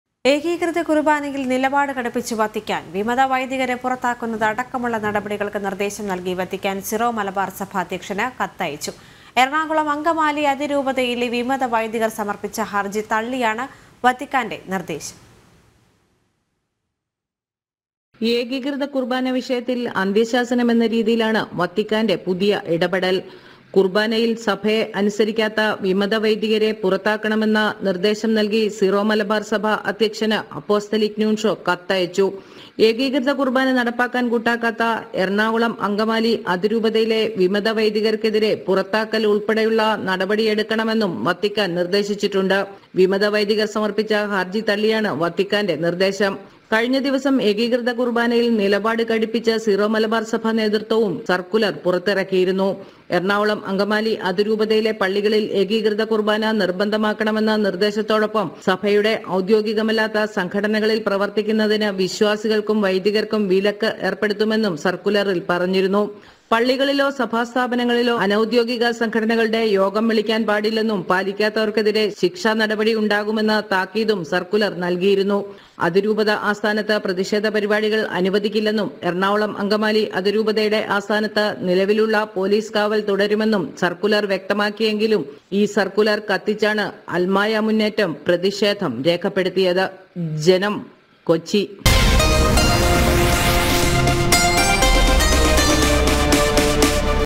ഏകീകൃത കുർബാനയിൽ നിലപാട് കടുപ്പിച്ച് വത്തിക്കാൻ | SABHA | ERNAKULAM
ഏകീകൃത  കുർബാനയിൽ നിലപാട് കടുപ്പിച്ച്  വത്തിക്കാൻ. വിമത വൈദികരെ പുറത്താക്കുന്നത് അടക്കമുള്ള നടപടികൾക്ക് നിർദേശം നൽകി വത്തിക്കാൻ സിറോ മലബാർ സഭ അധ്യക്ഷന് കത്ത് അയച്ചു. എറണാകുളം അങ്കമാലി അതിരൂപതയിലെ
വിമത വൈദികർ സമർപ്പിച്ച ഹർജി തള്ളിയാണ് വത്തിക്കാന്റെ നിർദേശം.